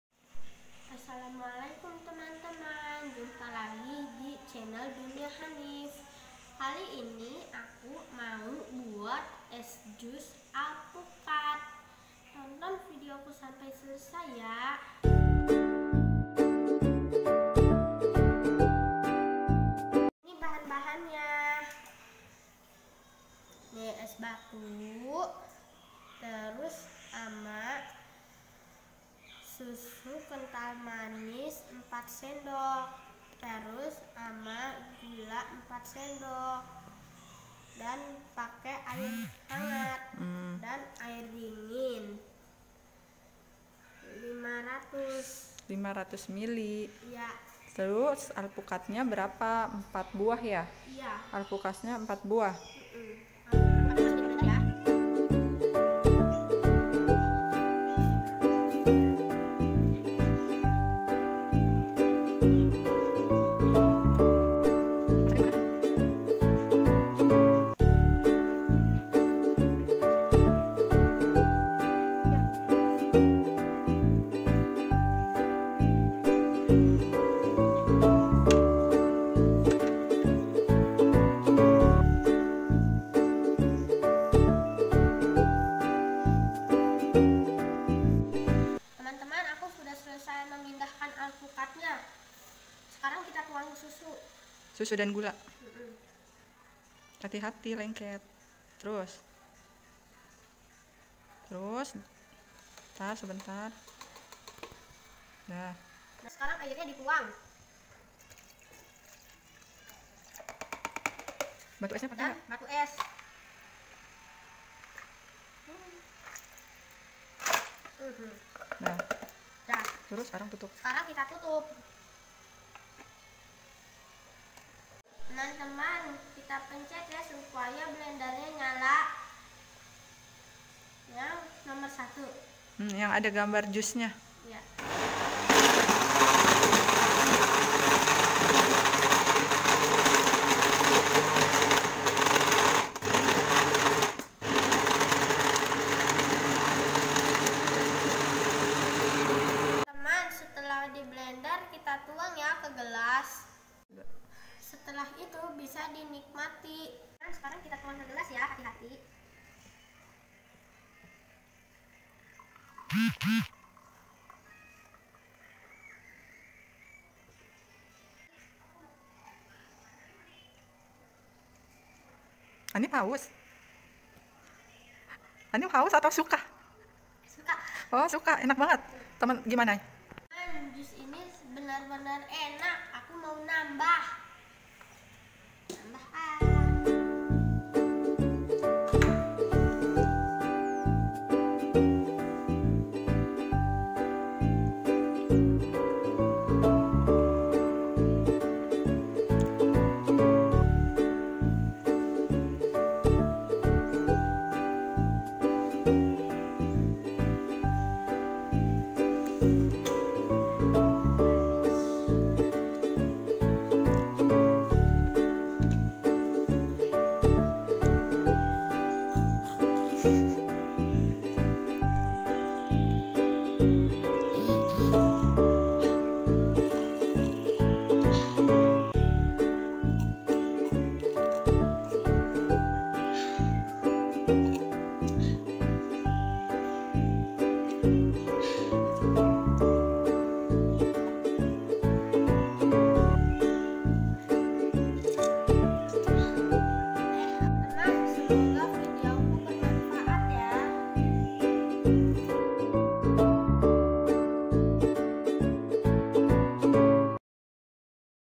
CARA MEMBUAT ES JUS ALPUKAT | FUN COOKING
Salah satu kegiatan belajar di rumah dengan tema Fun Cooking. Kebetulan ada stok buah alpukat, langsung deh di bikin minuman jus buah yang simpel. Tapi rasa tidak kalah nikmat, segar, manis, dan lembut.

Setelah selesai langsung habis dinikmati,,,,